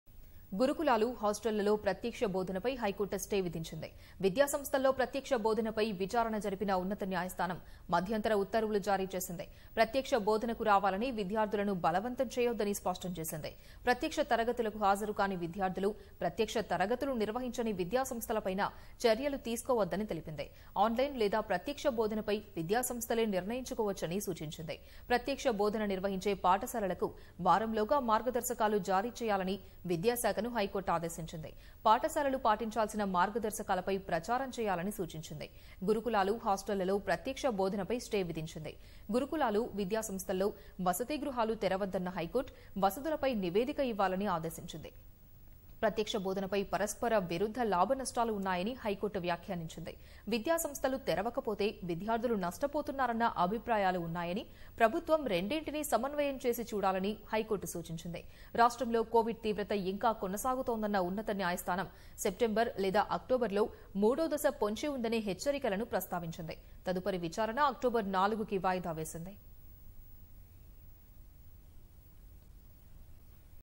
విద్యాసంస్థల్లో ప్రత్యక్ష బోధనపై హైకోర్టు మధ్యంతర ఉత్తర్వులు | HC Issues Interim Orders
గురుకులాలు, హాస్టళ్లలో ప్రత్యక్ష బోధనపై హైకోర్టు స్టే విధించింది. విద్యా సంస్థల్లో ప్రత్యక్ష బోధనపై విచారణ జరిపిన ఉన్నత న్యాయస్థానం మధ్యంతర ఉత్తర్వులు జారీచేసింది. ప్రత్యక్ష బోధనకు రావాలని విద్యార్థులను బలవంతం చేయొద్దని స్పష్టం చేసింది. ప్రత్యక్ష తరగతులు హాజరుకాని విద్యార్థులు, ప్రత్యక్ష తరగతులు నిర్వహించని విద్యా సంస్థలపైనా చర్యలు తీసుకోవద్దని తెలిపింది.